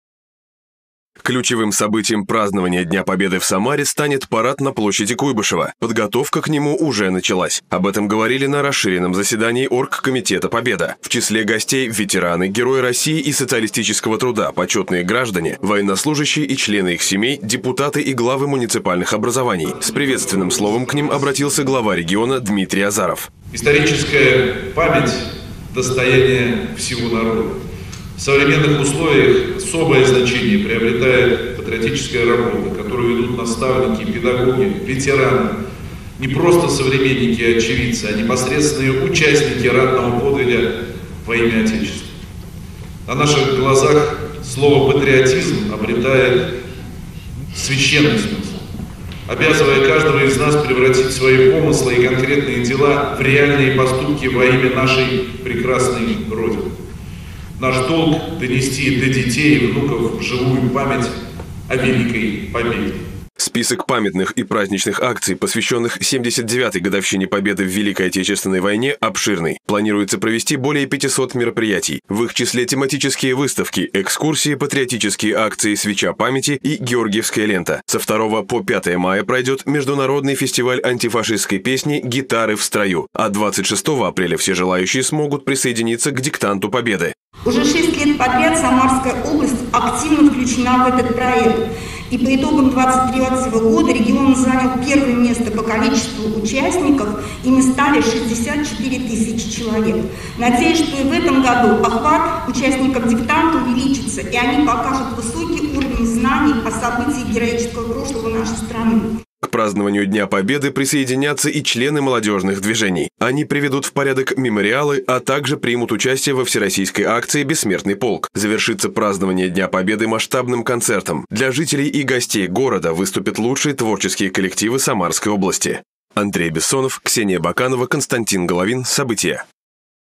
В Самаре состоялось заседание областного оргкомитета "Победа"
В Самаре прошло расширенное заседание оргкомитета "Победа". Его провел губернатор Самарской области Дмитрий Азаров. Главная тема встречи - подготовка к празднованию 79-й годовщины Победы в Великой Отечественной войне. Участники обозначили задачи. Подробности расскажем в сюжете.